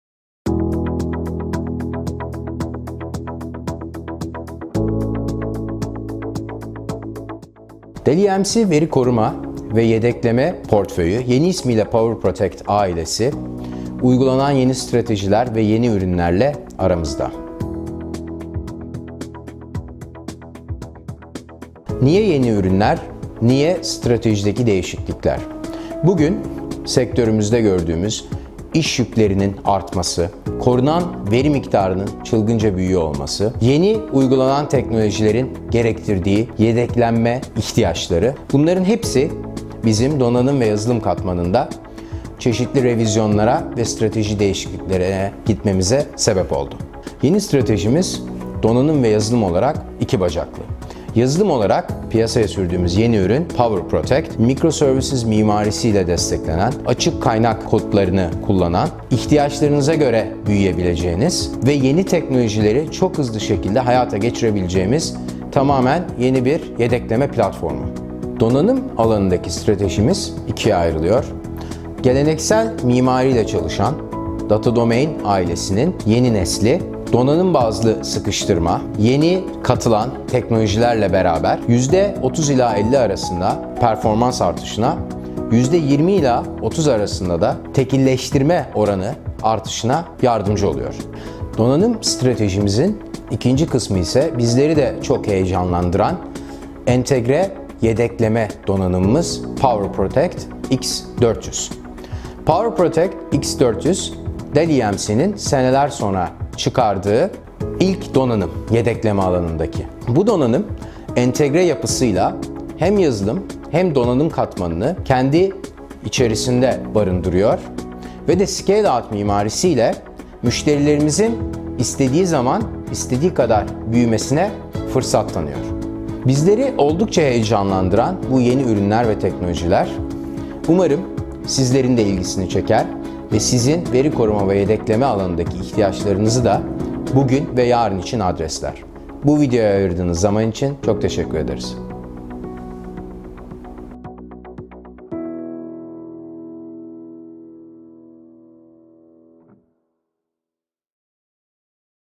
Dell Technologies - PowerProtect
Dell Technologies, Country Sales Manager - Şevket Kaan Ağaoğlu'nun anlatımıyla, PowerProtect ile ilgili merak ettiklerinizi açıklamaya çalıştık.